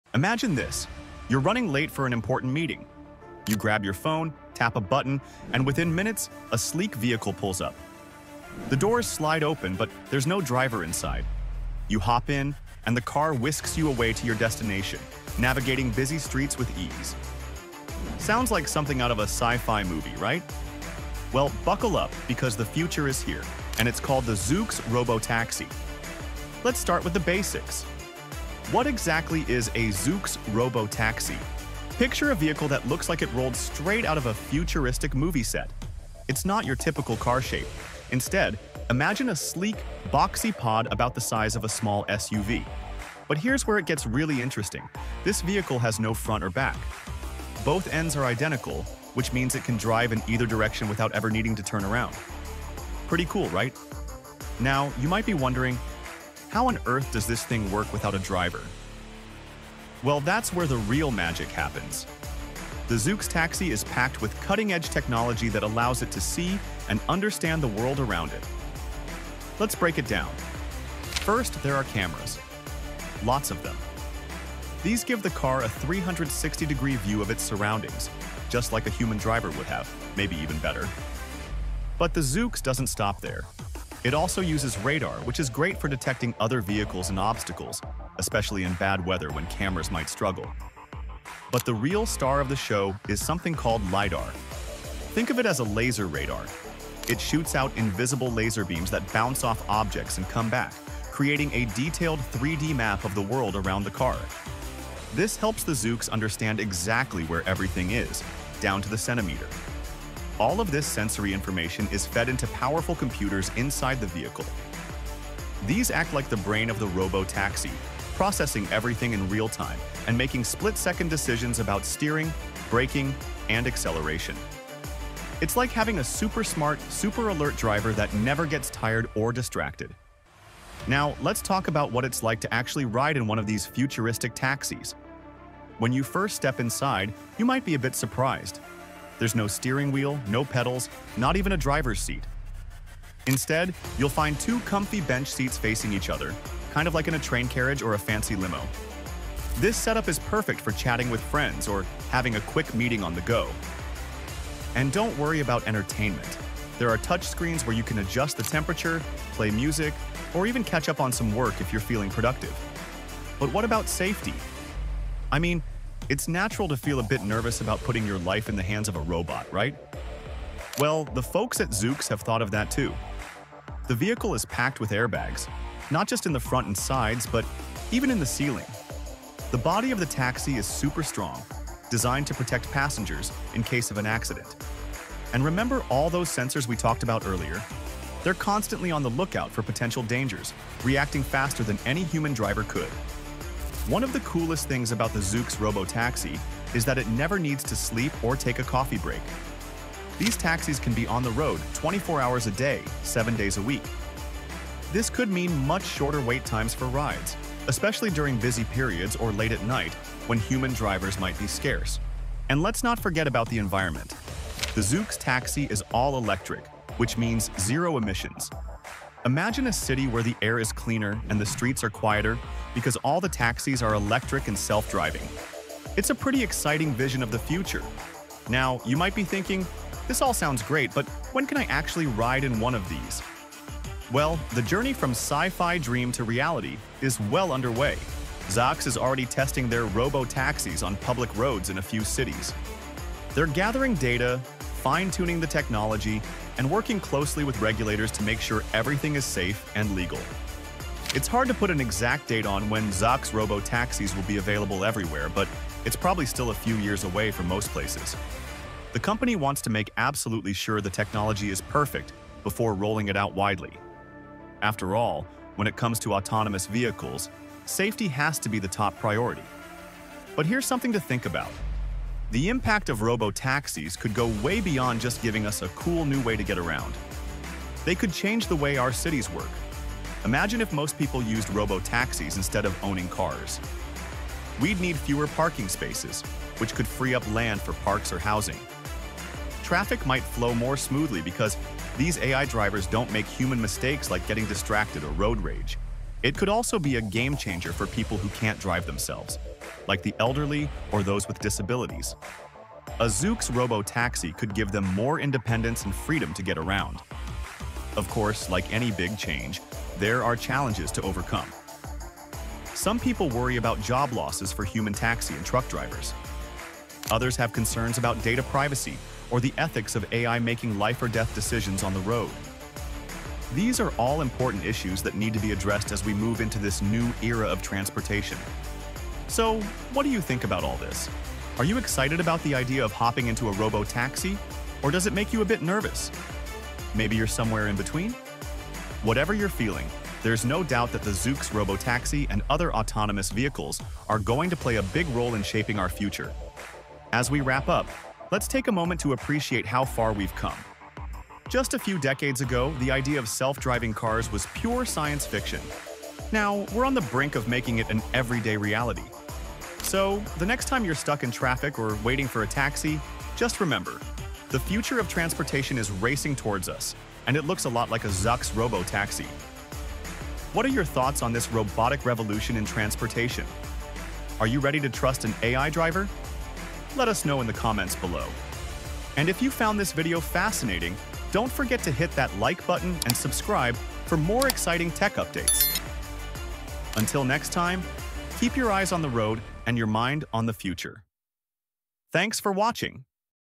No Steering Wheel, No Problem | The Incredible Zoox Robo Taxis
Welcome to the future of transportation! In this video, we dive into Zoox's revolutionary robotaxi—a fully autonomous vehicle with no steering wheel and no driver. We explore how Zoox Robo Taxis are changing the way we think about transportation, offering a glimpse into the future of self-driving cars. These innovative vehicles are designed to be fully autonomous, featuring cutting-edge technology and a unique design that sets them apart from traditional taxis.

Zoox is set to disrupt the world of ride-sharing and autonomous vehicles, and in this video, we break down the technology, benefits, and the potential future of robotaxis in cities around the world.

Time Stamps:
(0:02) The Future of Taxis  No Driver Needed!
(0:26) What Makes the Zoox Robo Taxi Unique
(1:02) How Does the Zoox Robo Taxi Work
(2:11) Inside the Zoox Robo Taxi- No Steering Wheel!
(2:44) Is the Zoox Robo Taxi Safe
(3:15) 24/7 Rides with Zoox Robo Taxi!
(3:35) Environment impact
(5:13) Challenges of Adopting Robo Taxis

What do you think about a taxi with no steering wheel? Could Zoox become the future of ride-sharing?